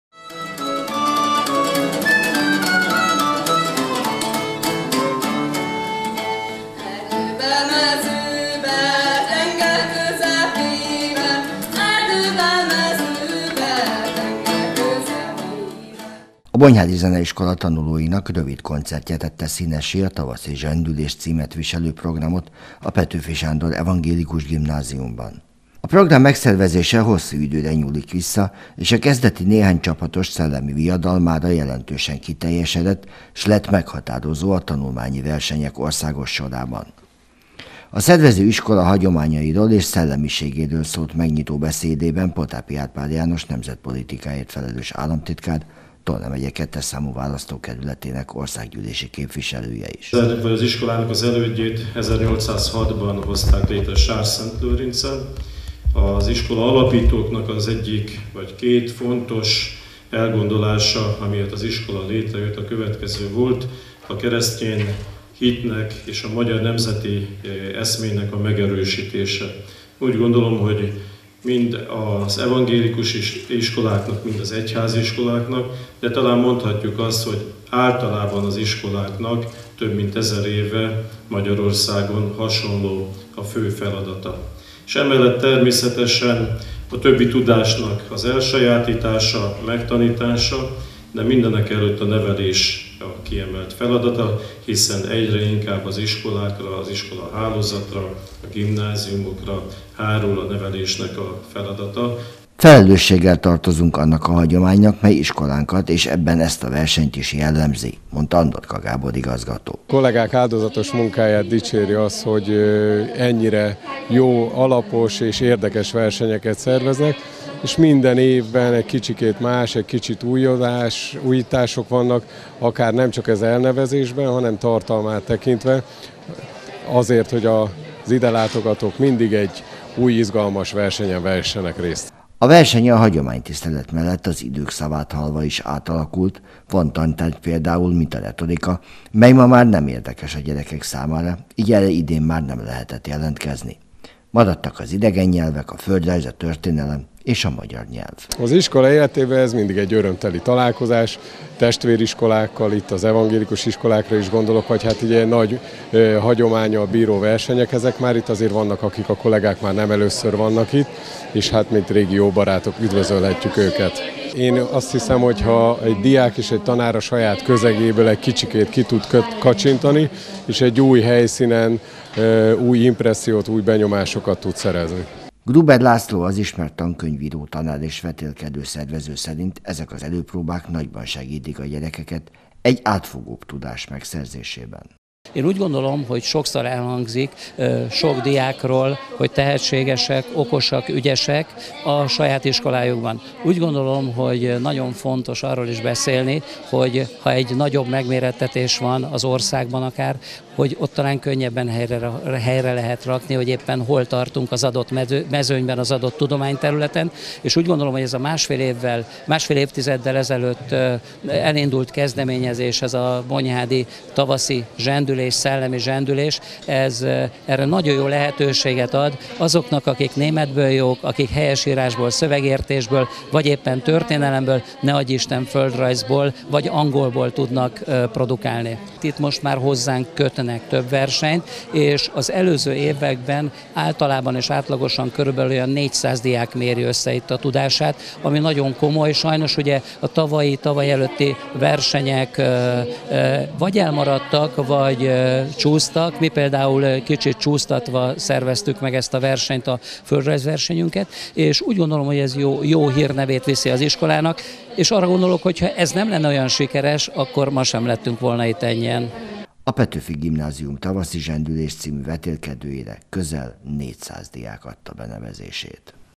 Nagyszabású tanulmányi verseny a bonyhádi Petőfi Gimnáziumban
Tavaszi zsendülés. Ezt a nevet viseli az a nagyszabású tanulmányi verseny ,melyet évről évre rendez meg a Bonyhádi Petőfi Sándor Evangélikus Gimnázium. Ez esemény, melyre az országból és a határon túlról is érkeznek versenyző csapatok részben hozzájárul a tanulók tudásának bővítéséhez, de egyben emeli is az iskola ázsióját .